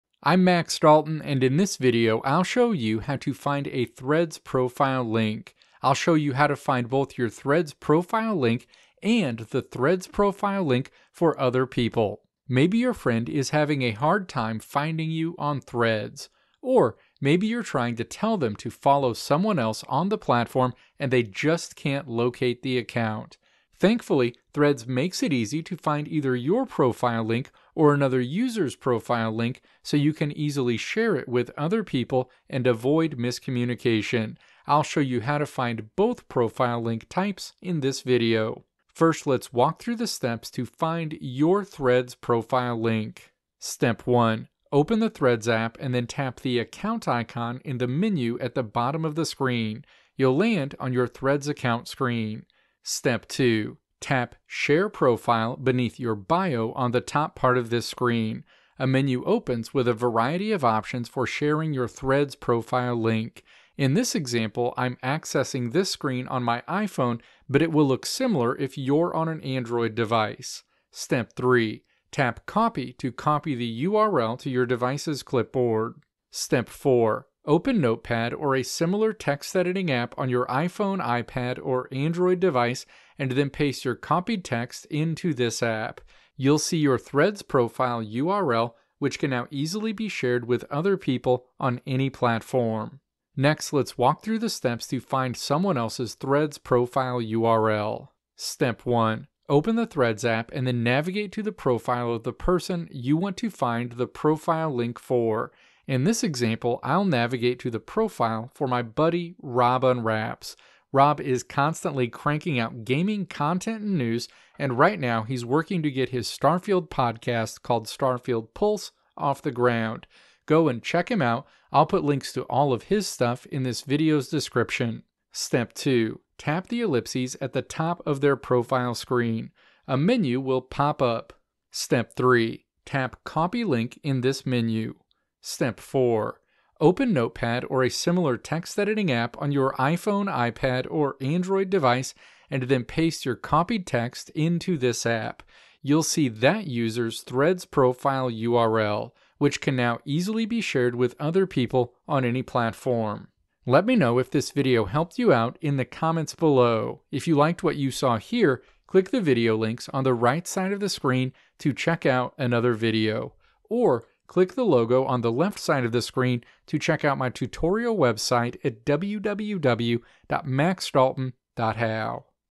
How to Find a Threads Profile Link
In this video I'll show you how to find a Threads profile link. I'll show you how to find both your Threads profile link, and the Threads profile link for other people.

Timestamps:
Introduction: 0:00
Steps to Find Your Threads Profile Link: 0:38
Steps to Find Someone Else's Threads Profile URL: 1:33
Conclusion: 2:33

Video Transcript:
Maybe your friend is having a hard time finding you on Threads. Or, maybe you're trying to tell them to follow someone else on the platform, and they just can't locate the account. Thankfully, Threads makes it easy to find either your profile link or another user's profile link, so you can easily share it with other people and avoid miscommunication. I'll show you how to find both profile link types in this video.

First let's walk through the steps to find your Threads profile link.

Step 1. Open the Threads app, and then tap the "Account" icon in the menu at the bottom of the screen. You'll land on your Threads account screen.

Step 2. Tap "Share Profile" beneath your bio on the top part of this screen. A menu opens with a variety of options for sharing your Threads profile link. In this example I'm accessing this screen on my iPhone, but it will look similar if you're on an Android device.

Step 3. Tap "Copy" to copy the URL to your device's clipboard.

Step 4. Open Notepad or a similar text editing app on your iPhone, iPad or Android device, and then paste your copied text into this app. You'll see your Threads profile URL, which can now easily be shared with other people on any platform.

Next let's walk through the steps to find someone else's Threads profile URL.

Step 1. Open the Threads app, and then navigate to the profile of the person you want to find the profile link for. In this example I'll navigate to the profile for my buddy Rob Unwraps. Rob is constantly cranking out gaming content and news, and right now he's working to get his Starfield podcast called Starfield Pulse off the ground. Go and check him out, I'll put links to all of his stuff in this video's description.

Step 2. Tap the ellipses at the top of their profile screen. A menu will pop up.

Step 3. Tap "Copy Link" in this menu.

Step 4. Open Notepad or a similar text editing app on your iPhone, iPad or Android device, and then paste your copied text into this app. You'll see that user's Threads profile URL, which can now easily be shared with other people on any platform.